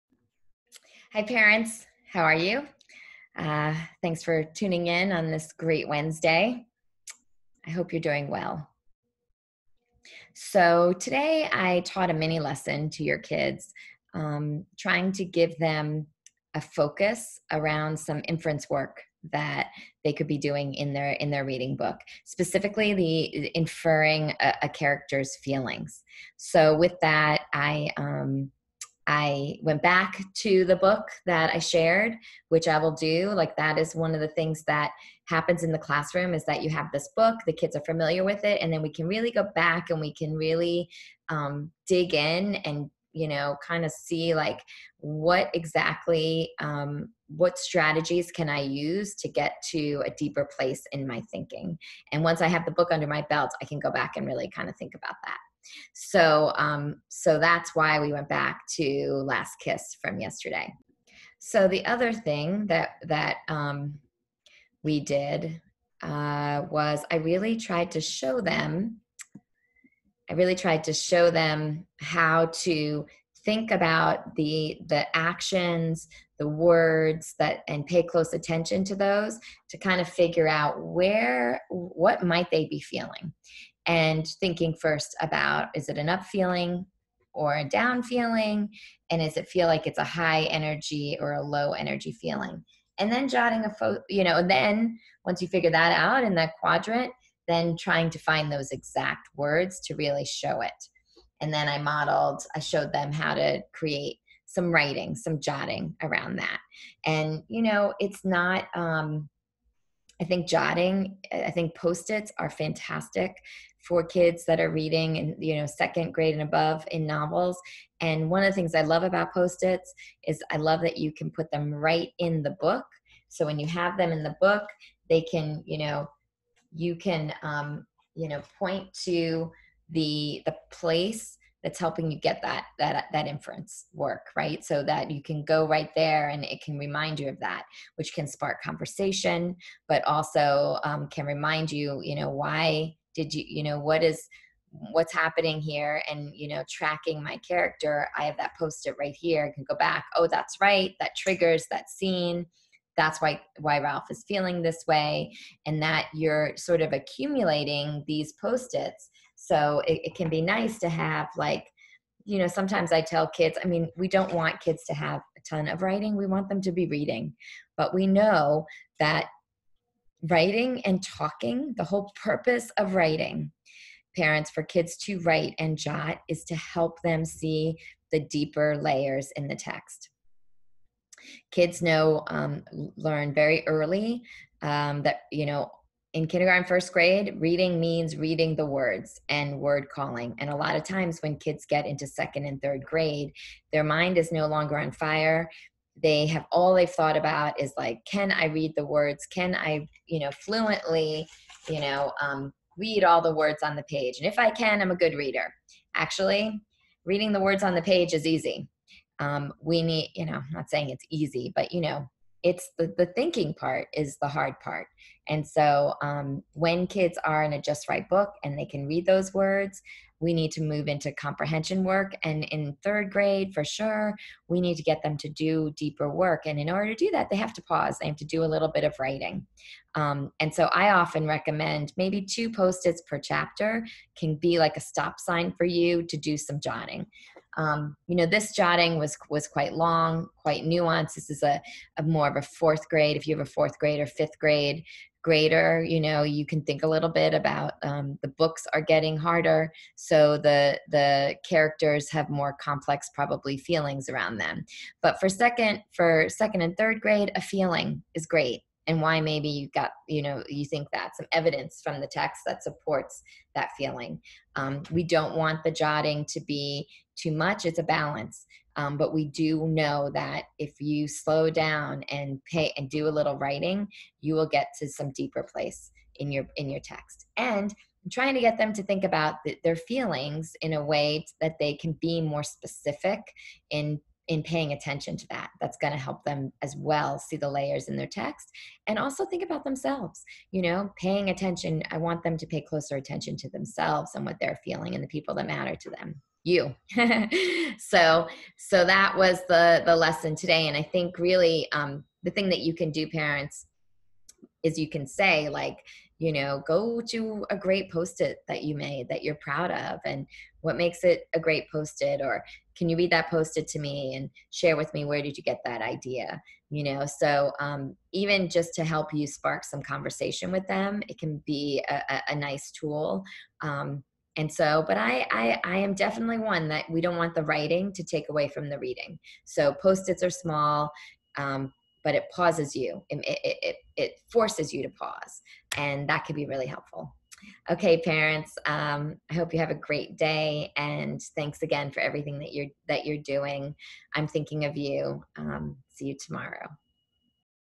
parents day 3 reading ML
This video will give your kids a strategy to help with inferring.  It will also give them direction on how to look across a text and get these bigger ideas.  And It will also give them ideas on what to write to help develop their thinking.